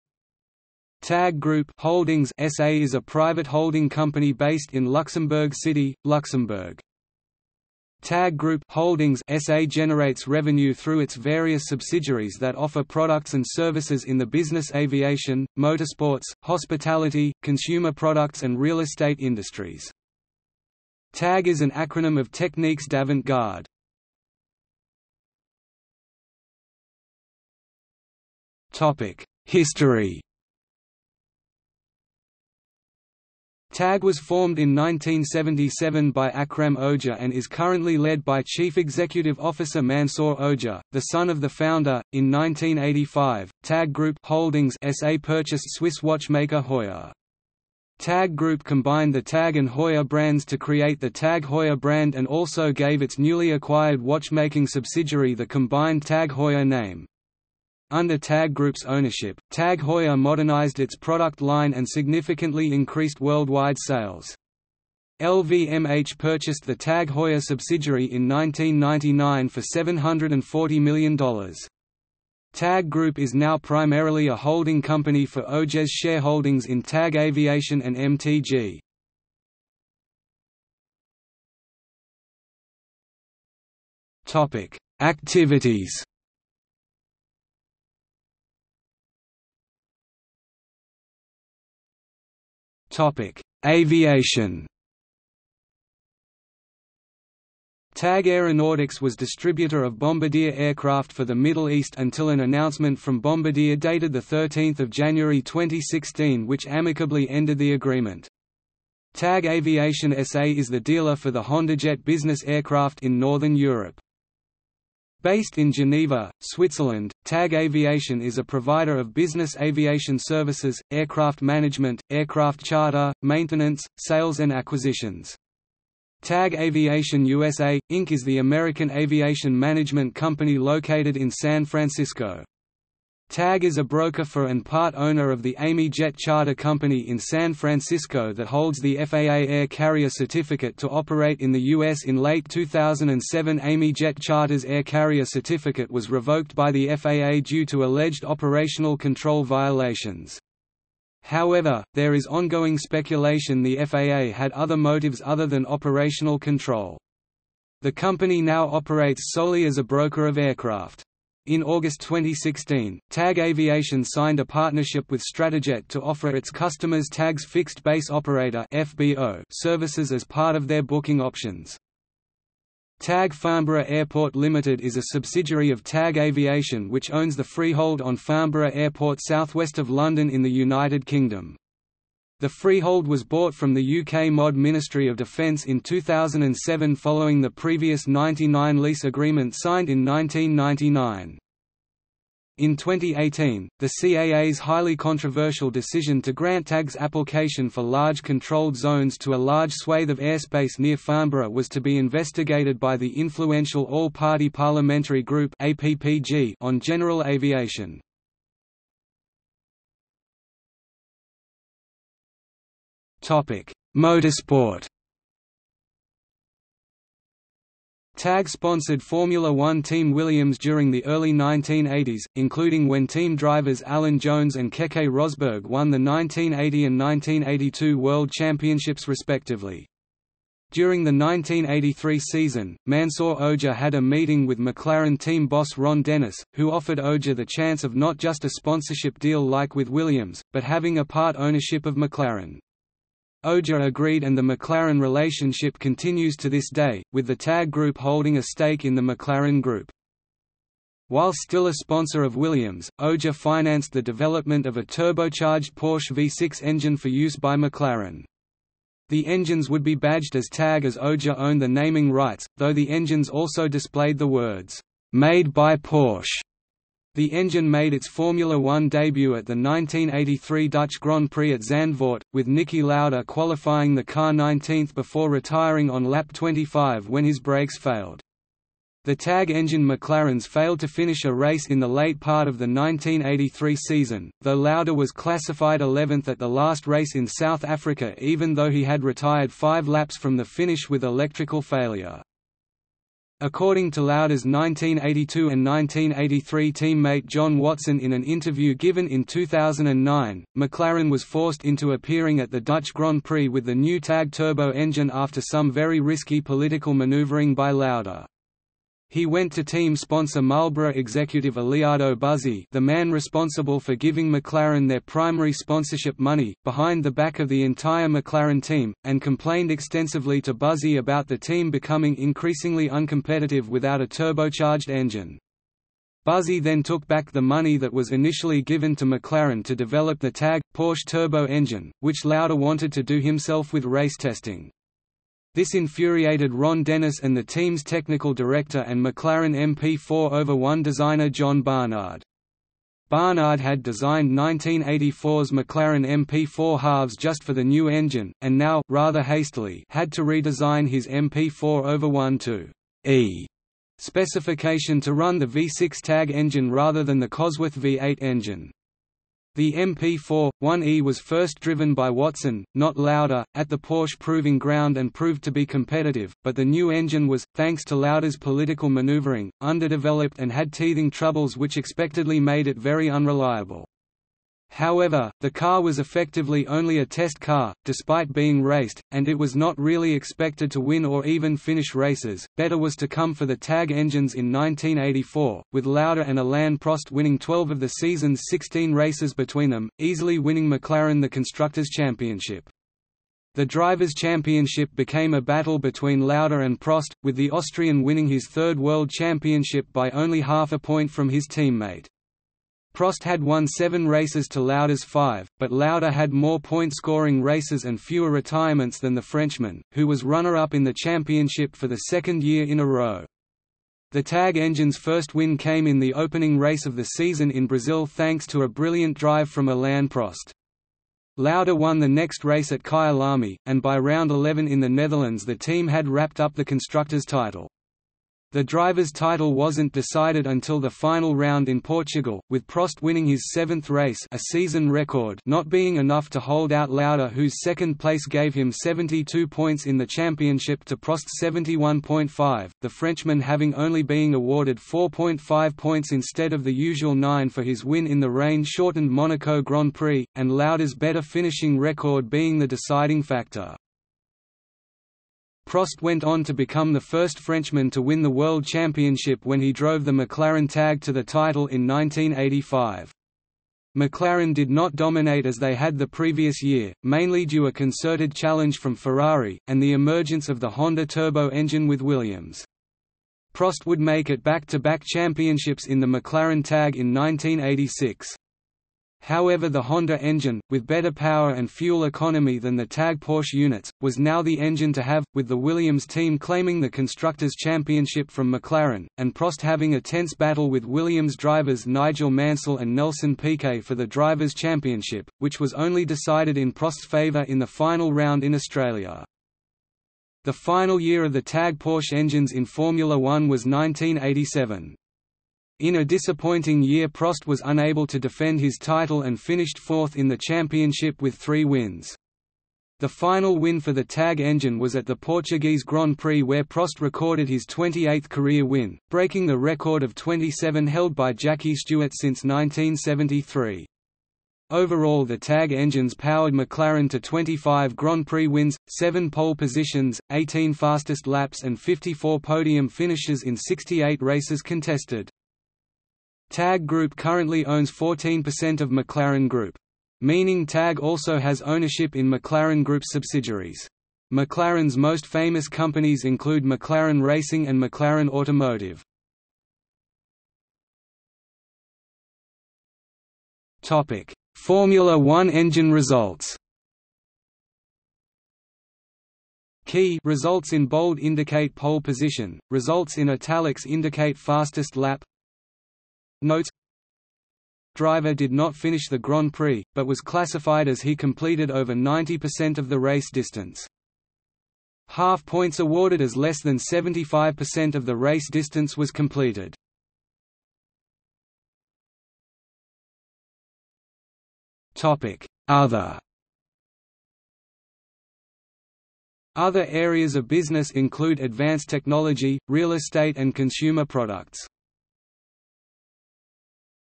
Techniques d'Avant Garde | Wikipedia audio article
00:00:25 1 History
00:01:20 2 Activities
00:01:29 2.1 Aviation
00:03:30 2.2 Motorsport
00:09:10 2.3 Formula One engine results
00:09:41 2.4 Other

- Socrates

SUMMARY
TAG Group (Holdings) S.A. is a private holding company based in Luxembourg City, Luxembourg.
TAG Group (Holdings) S.A. generates revenue through its various subsidiaries that offer products and services in the business aviation, motorsports, hospitality, consumer products and real estate industries.
TAG is an acronym of Techniques d'Avant Garde.